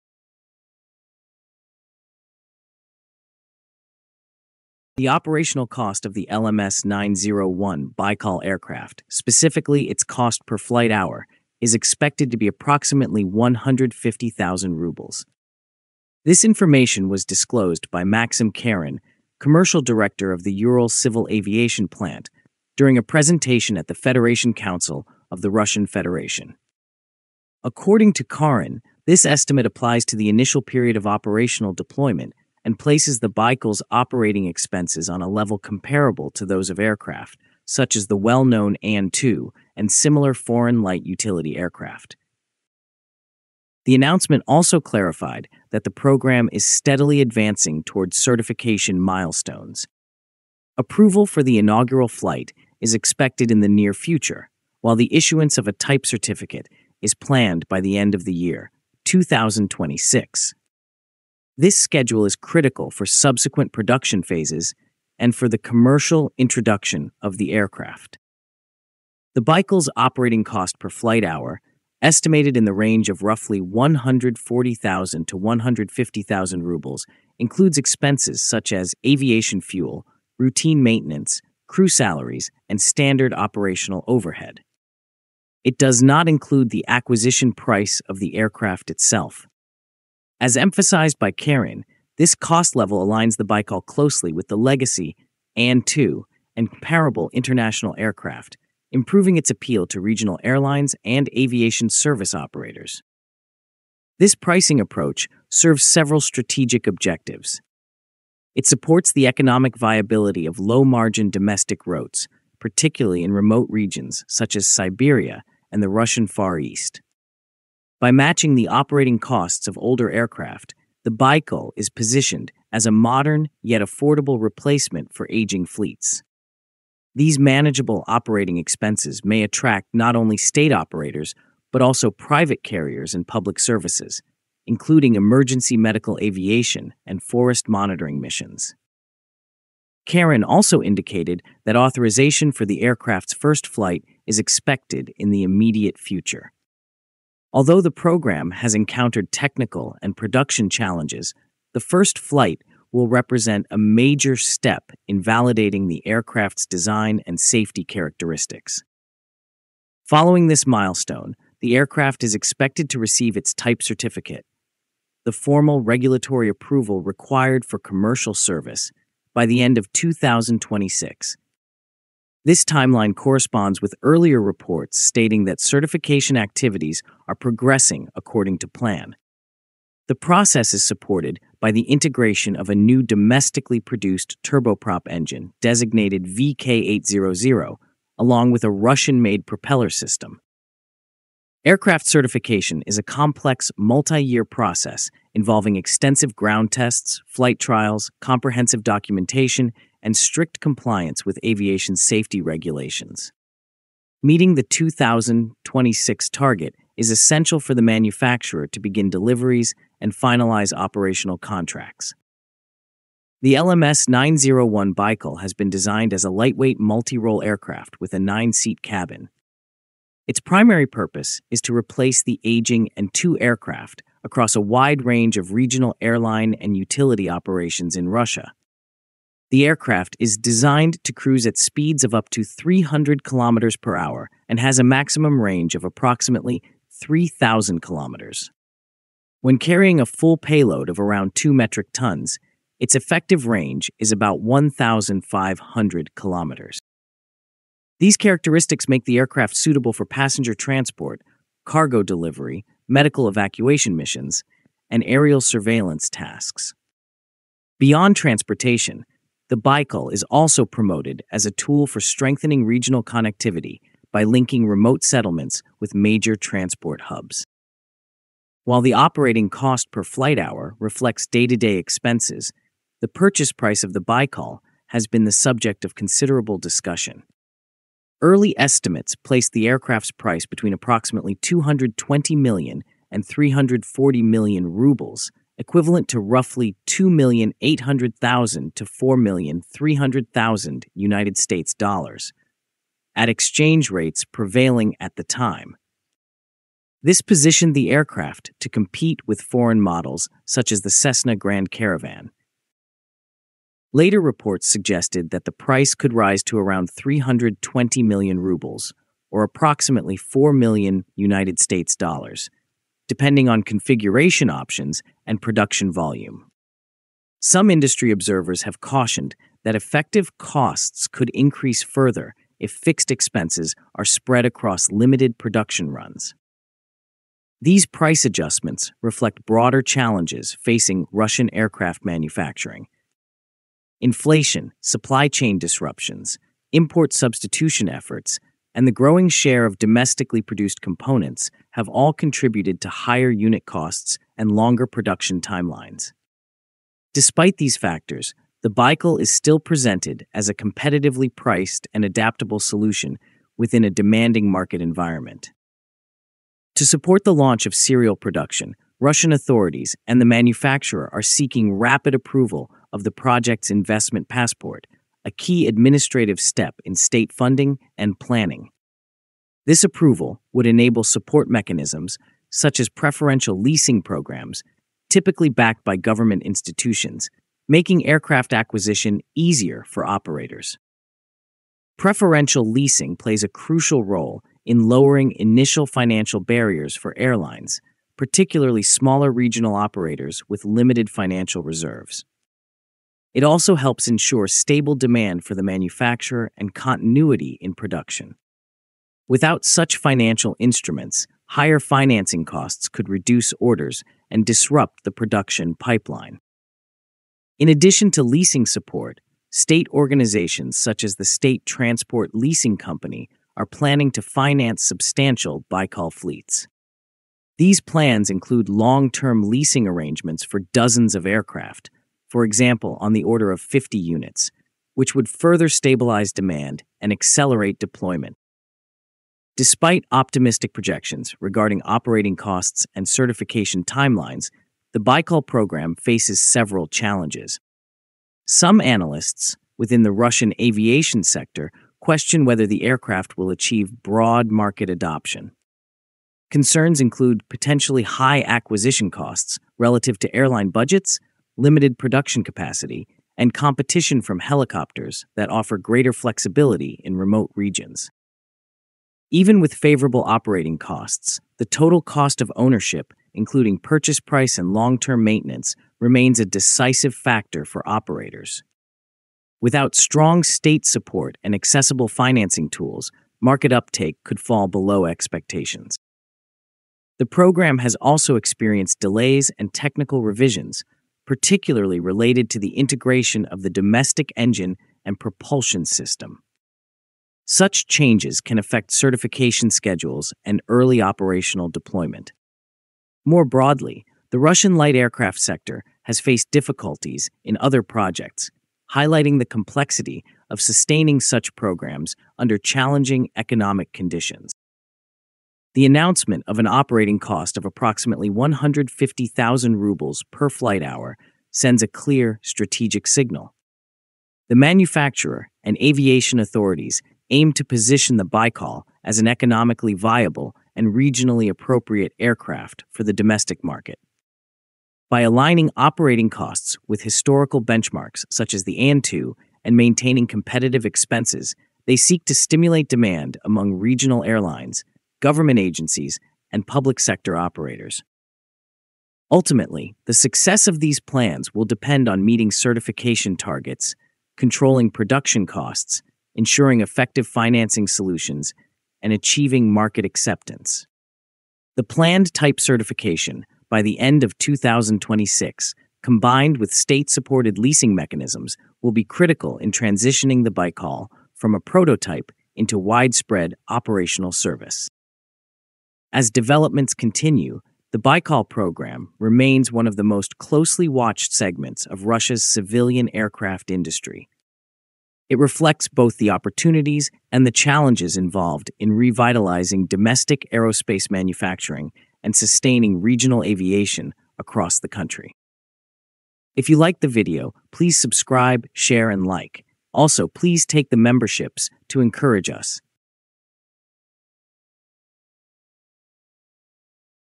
Russia’s Baikal Aircraft: Per Flight Hour is Same as AN-2 | First Flight Soon
Russia’s new LMS-901 “Baikal” aircraft is approaching a critical milestone. According to UZGA Commercial Director Maxim Kharin, the cost of a single flight hour will be around 150,000 rubles, placing it on par with the legendary An-2 and comparable foreign aircraft. Speaking at the Federation Council, Kharin confirmed that approval for Baikal’s first flight is expected soon, while full type certification is planned by the end of 2026.

The Baikal is designed to revive regional aviation across Russia, especially in remote and sparsely populated regions. However, for serial production to begin on time, officials stress the urgent need to approve the project’s investment passport. This would unlock preferential leasing schemes, allowing airlines to acquire the aircraft at reduced financial risk.

In this video, we break down Baikal’s operating costs, certification timeline, financing challenges, and whether it can truly become Russia’s long-awaited replacement for the An-2.